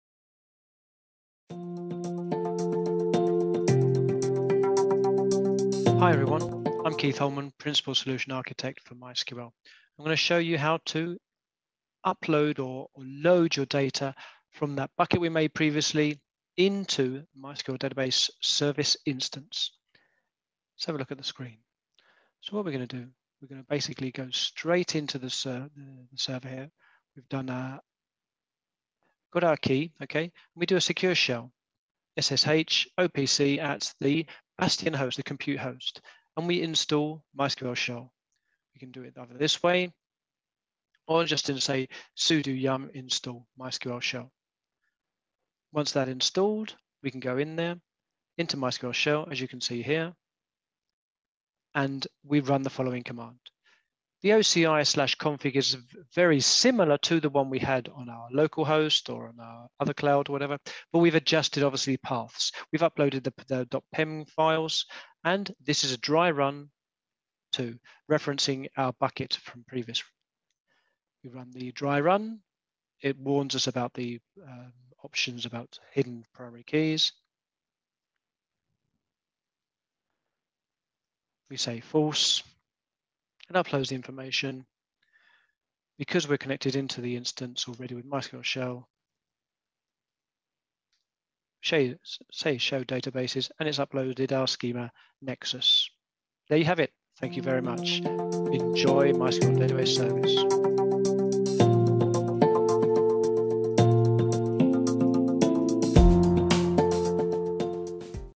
How to Load Your Data to MySQL Database Service
Once you have exported your MySQL old data, you just need to load the exported database file from the object storage bucket on Oracle Cloud to your new MySQL Database Service instance.